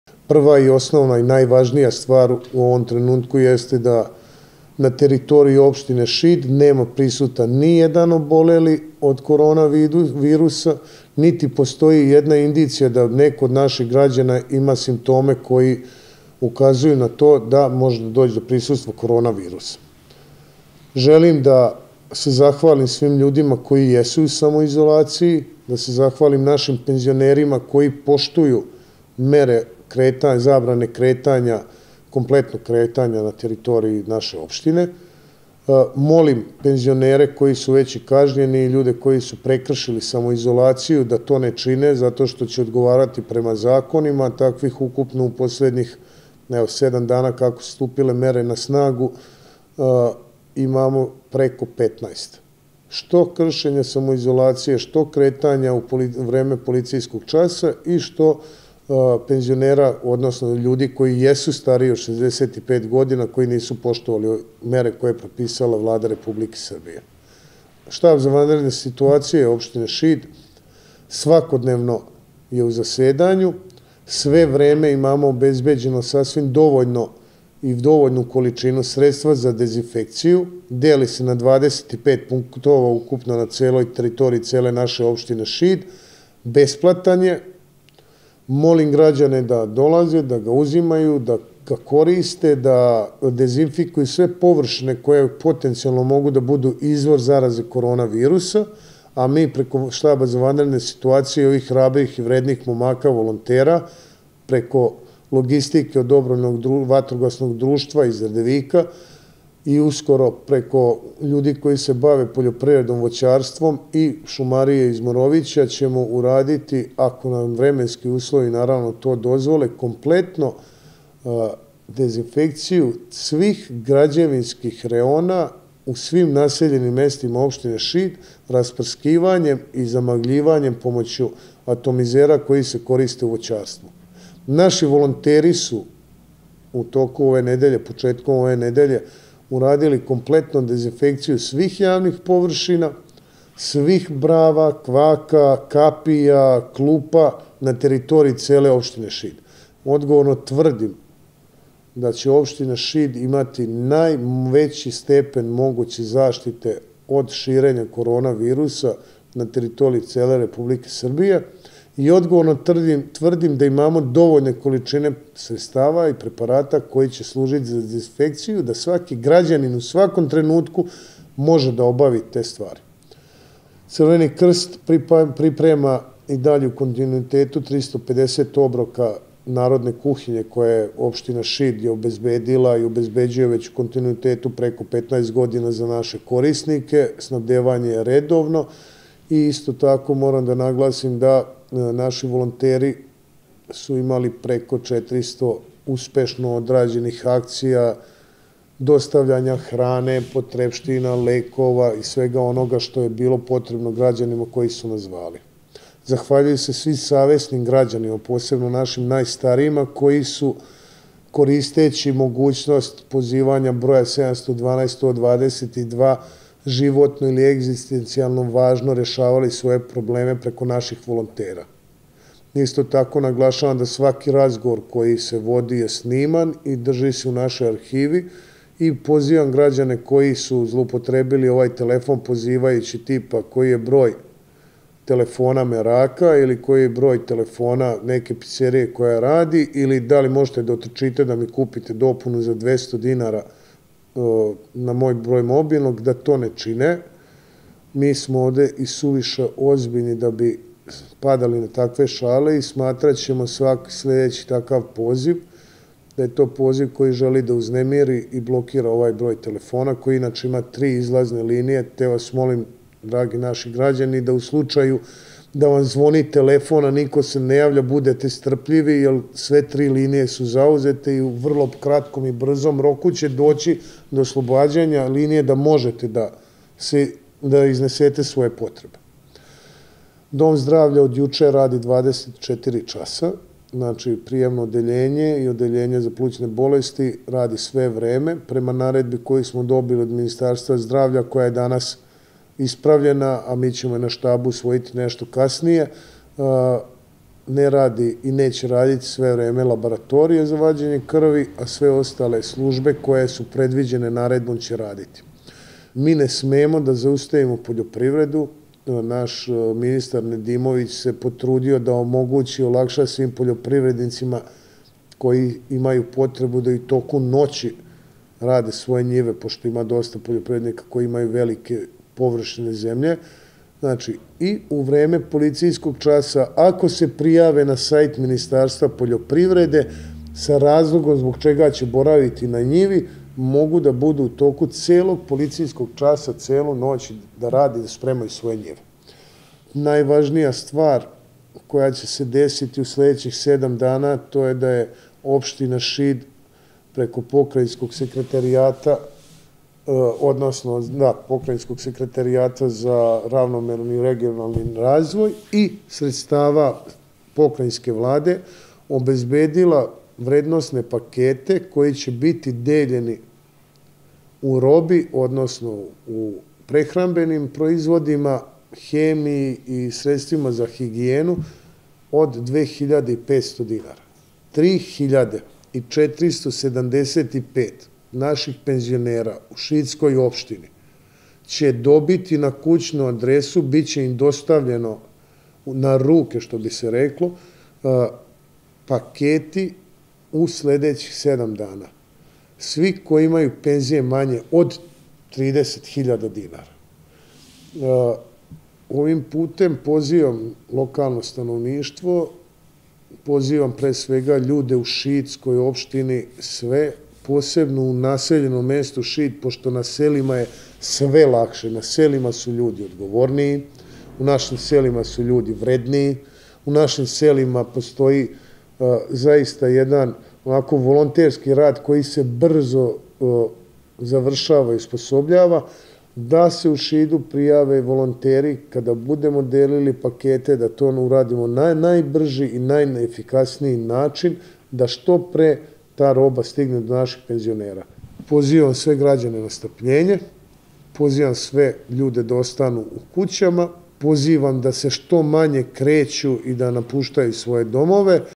Konferencija za novinare Štaba za vanredne situacije opštine Šid
Komandant štaba za vanredne situacije u Opštini Šid, Zoran Semenović danas je održao konferenciju za novinare kako bi obavestio građane o svim merama i akcijama koje se sprovode na teritoriji naše opštine, s ciljem prevencije Korona virusa ili kao volonterska aktivnost pri pomoći svima kojima je to potrebno. Za sada u našoj opštini nema obolelih od Korona virusa, a jedan broj građana nalazi se u samoizolaciji. Novina je da od sada Ambulanta za respiratorne infekcije Doma zdravlja Šid radi 24 sata.